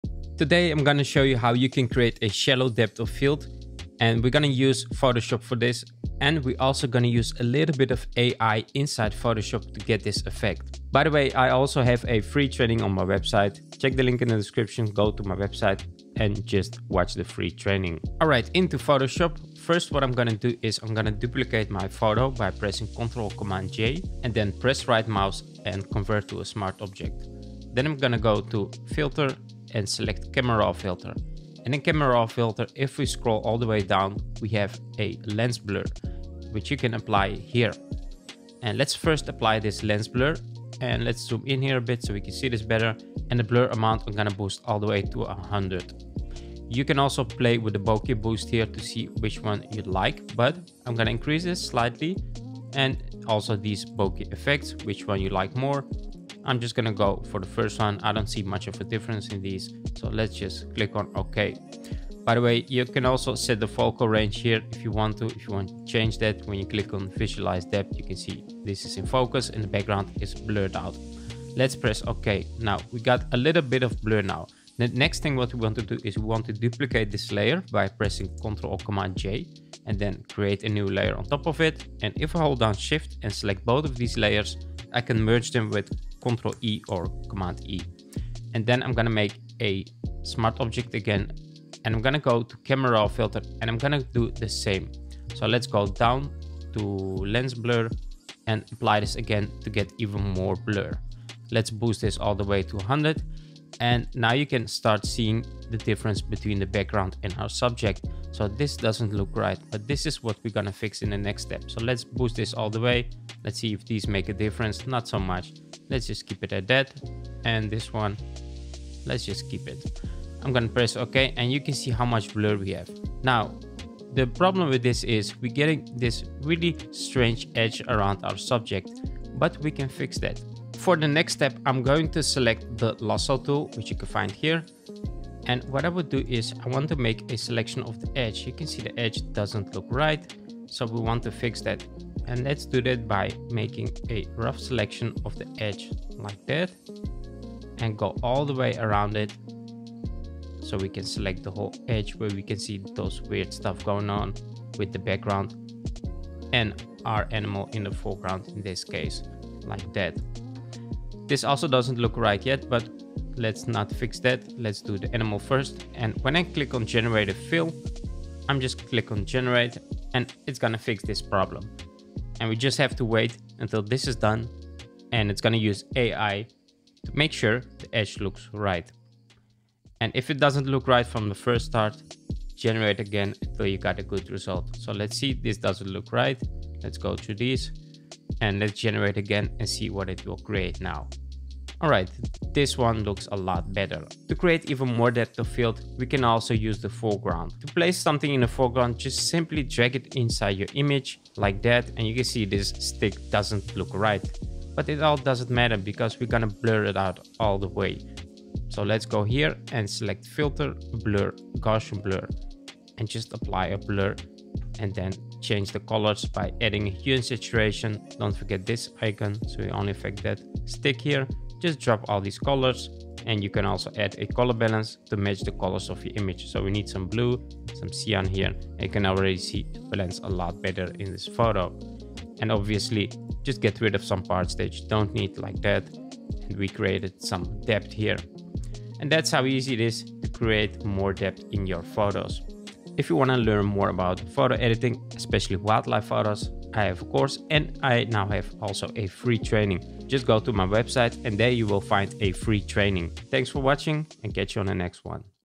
How to Create a Shallow Depth of Field in Photoshop
Create a blurry background in Adobe Photoshop with this tutorials and use Generative Fill AI to get even better results.